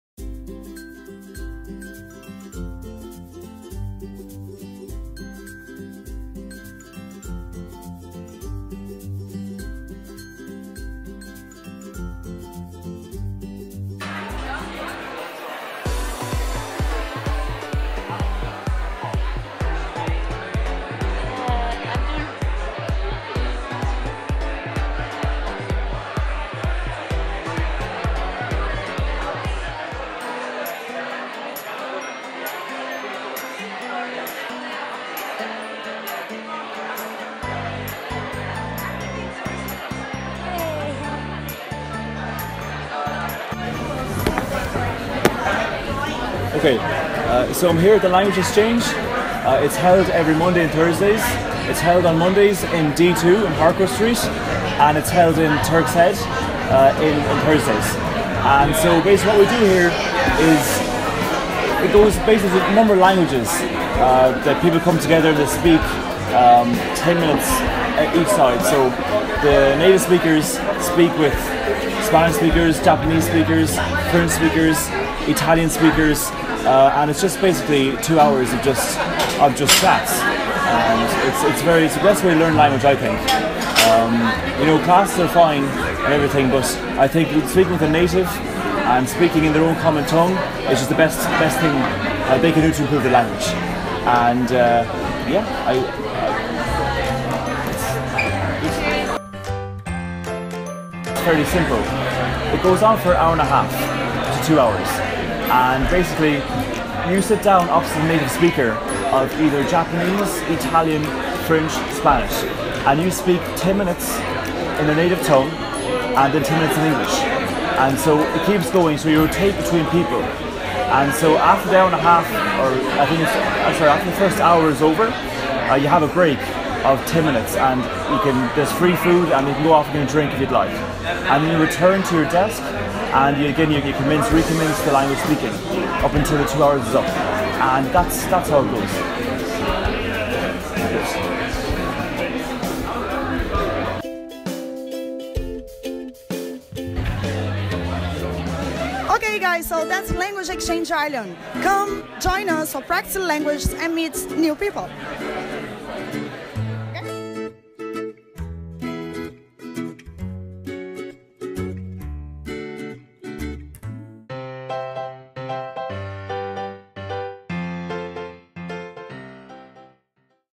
Turk's Head Language Exchange
Every Thursday at 6.30 a language exchange takes place at the Turk's Head, 2 minutes walk from DBS.
That is a great experience to learn new languages and meet new people from different cultures!